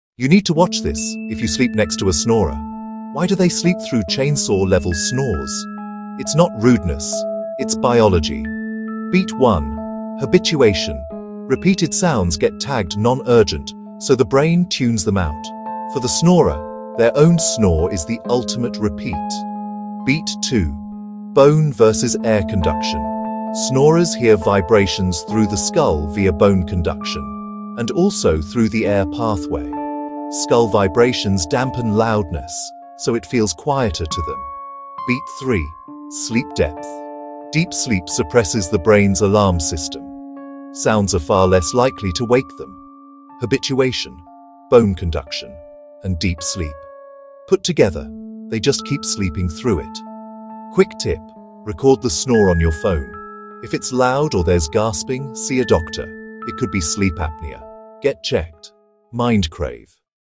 Why Your Own Snore NEVER Wakes You Up 😴
Ever been told you were snoring like a freight train, but you didn't hear a thing? 🤔 It's not your hearing, it's your brain!

During sleep, your brain activates a "mute" feature for predictable, self-generated sounds so you can stay asleep. It knows the sound isn't a threat! Pretty smart, right? 🧠

Tag someone whose snoring DEFINITELY wakes YOU up! 😂